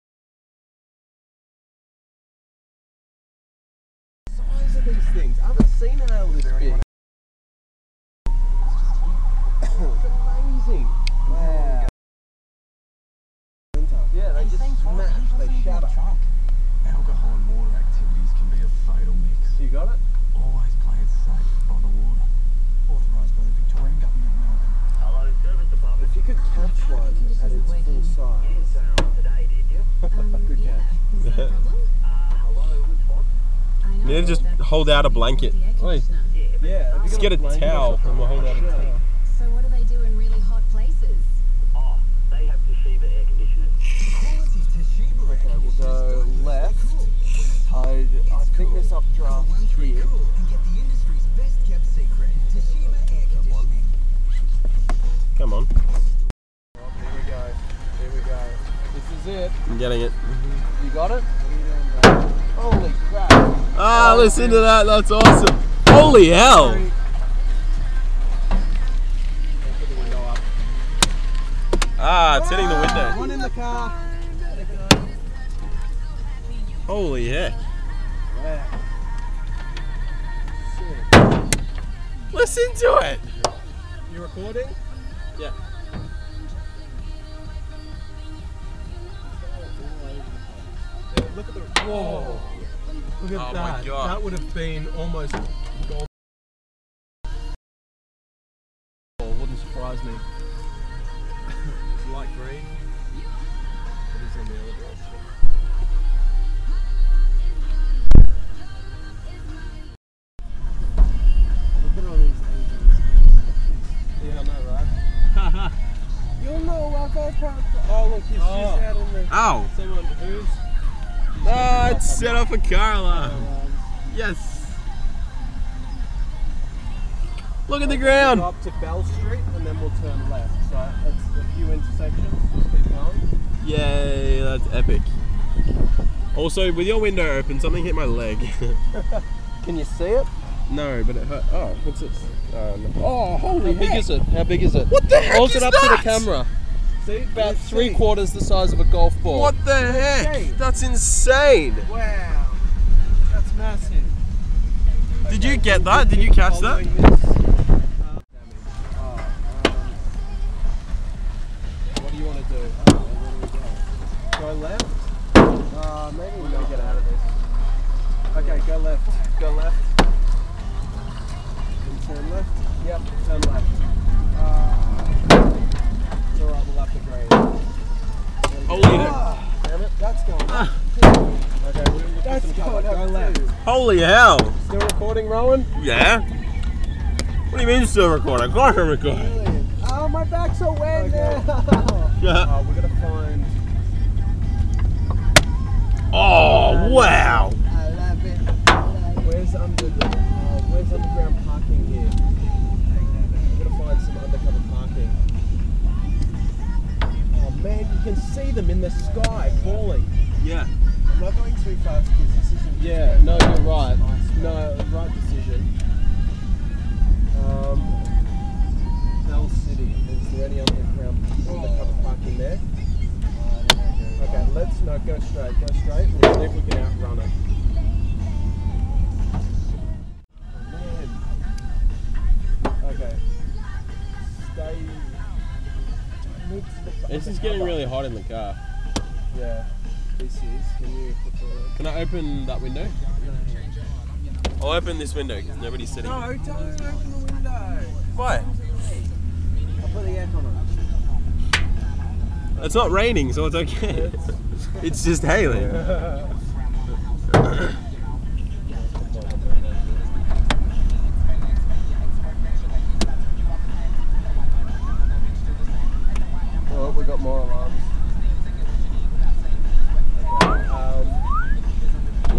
Hail in Preston
Driving through Preston, under the Rain Free Base of a Cell as in came through the northern suburbs of Melbourne. Appears that this is the place to be for larger than normal hail. 25-12-11